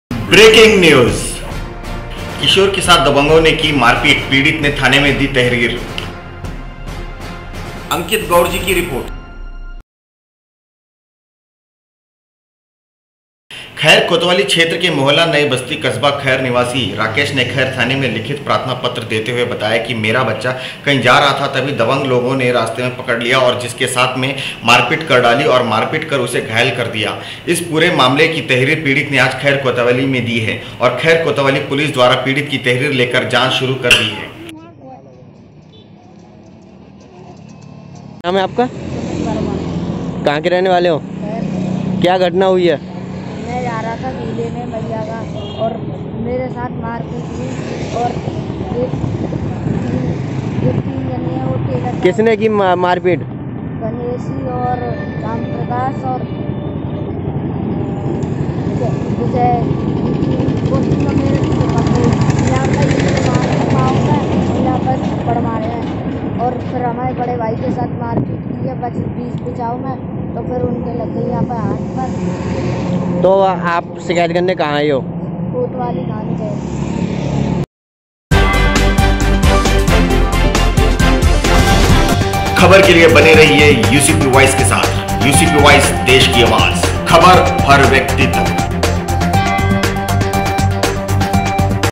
किशोर के साथ दबंगों ने की मारपीट पीड़ित ने थाने में दी तहरीर |khair aligarh uttarpradesh|ucp news
कस्बा के मोहल्ला नई बस्ती किशोर के साथ दबंगों ने की मारपीट पीड़ित ने थाने में दी तहरीर

Reporter : Ankit Gaur

 खैर कोतवाली क्षेत्र के मोहल्ला नई बस्ती कस्बा खैर निवासी राकेश ने खैर थाने में लिखित प्रार्थना पत्र देते हुए बताया कि मेरा बच्चा कहीं जा रहा था तभी दबंग लोगों ने रास्ते में पकड़ लिया और जिसके साथ में मारपीट कर डाली और मारपीट कर उसे घायल कर दिया |

इस पूरे मामले की तहरीर पीड़ित ने आज खैर कोतवाली में दी है और खैर कोतवाली पुलिस द्वारा पीड़ित की तहरीर लेकर जांच शुरू कर दी है|

*UCP Voice देश की आवाज़ *
FREE JOINING as UCP REPORTER
(News in Min. 5 & Max. 10 Platforms)
* ADS, Commercial News/Brands/Profession